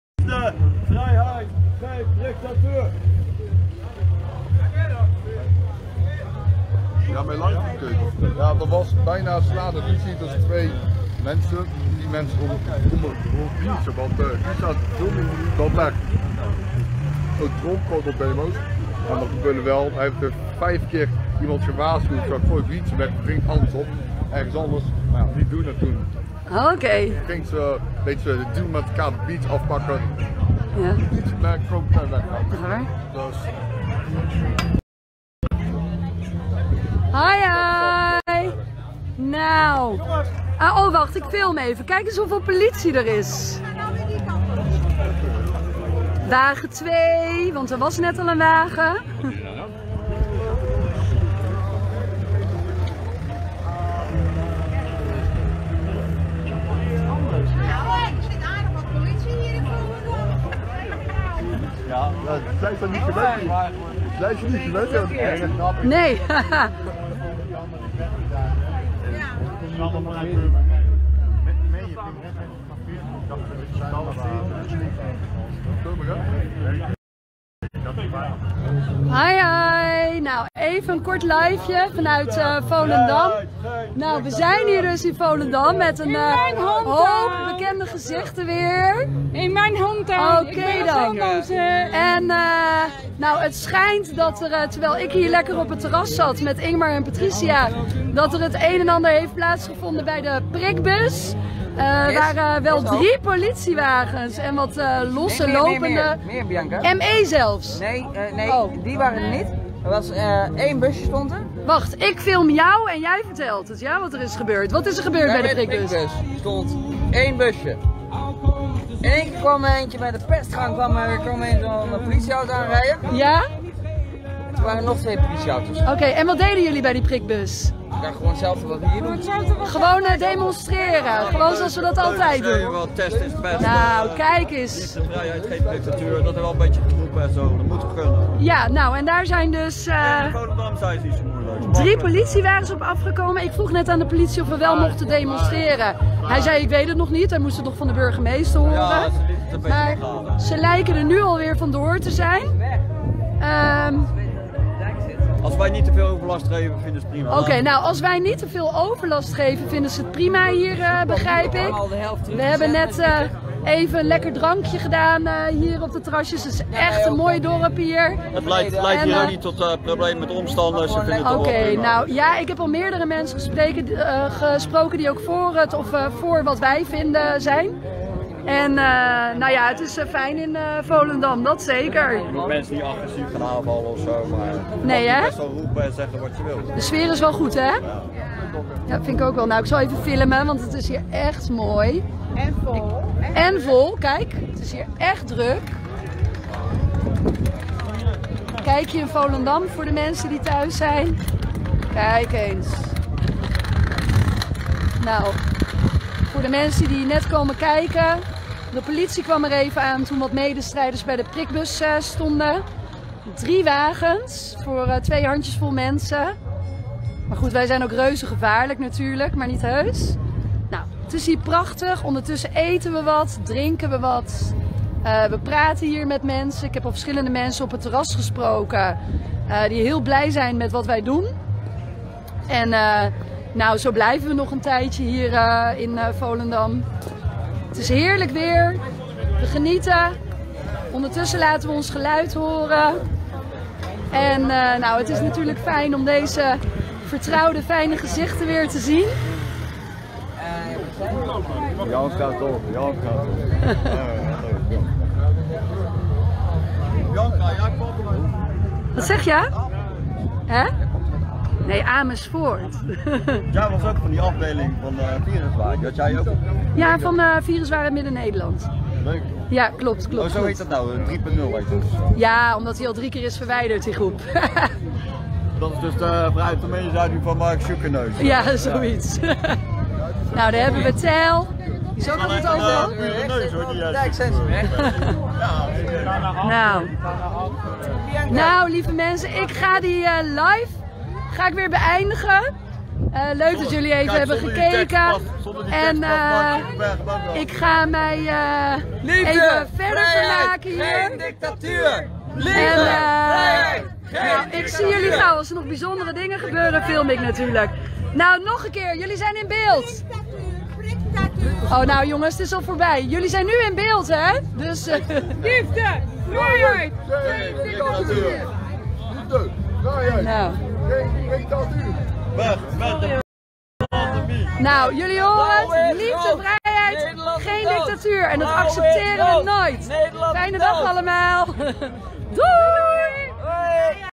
Volendam kreeg bezoek van Demonstranten - 24 oktober 2021
De zon scheen volop op deze fraaie herfstdag. Terwijl honderden mensen de marathon liepen in Rotterdam, brachten demonstranten een 'toeristisch' bezoek aan Volendam.
Beelden zijn afkomstig van een livestream op Facebook en gemaakt door Bianca Wagt.